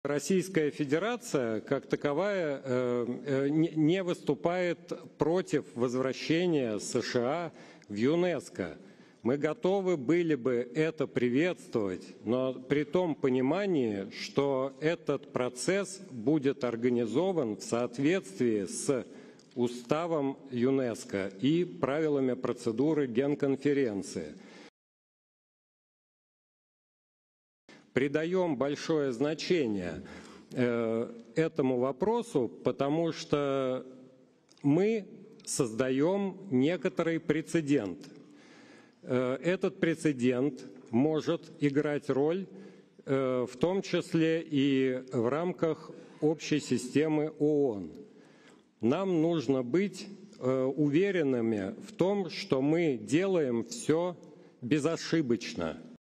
Russia not opposing US rejoining UNESCO
RESTRICTION SUMMARY:
ASSOCIATED PRESS
Paris - 29 June 2023
1. SOUNDBITE (Russian) Rinat Alyautdinov, Russia's Permanent Representative to UNESCO:
“The Russian Federation as such does not oppose the return of the United States to UNESCO. We would be ready to welcome this, but with the understanding that this process is organised in accordance with the UNESCO Charter and the rules of procedure of the General Conference.”
++BLACK FRAMES++
2. SOUNDBITE (Russian) Rinat Alyautdinov, Russia's Permanent Representative to UNESCO:
“We place a high priority to this issue because we are setting a certain precedent. This precedent can play a role, including within the framework of the UN common system. We need to be sure that we do everything without mistakes.”

STORYLINE:
Russia is not against the U.S. rejoining the United Nation's cultural organisation, Russia's Permanent Representative to UNESCO said on Thursday.

“We would be ready to welcome this, but with the understanding that this process is organised in accordance with the UNESCO Charter and the rules of procedure of the General Conference,” Rinat Alyautdinov said.

UNESCO’s 193 members states gathered on Thursday for a two-day meeting in Paris aimed at voting on the United States’ plans to rejoin the U.N. cultural and scientific agency after a decade-long dispute sparked by the organisation’s move to include Palestine as a member.

The U.S. announced earlier this month, that it wanted to return, five years after it withdrew from the agency during the presidency of Donald Trump.

U.S. officials say the decision was motivated by concern that China is filling the gap left by the U.S. in UNESCO policymaking, notably in setting standards for artificial intelligence and technology education around the world.

Approval by member states seems a formality since not a single country has raised an objection to the return of a country that was once the agency’s single biggest funder. The vote is expected Thursday or Friday.

The U.S. and Israel stopped financing UNESCO after it voted to include Palestine as a member state in 2011. The Trump administration decided in 2017 to withdraw from the agency altogether the following year, citing long-running anti-Israel bias and management problems.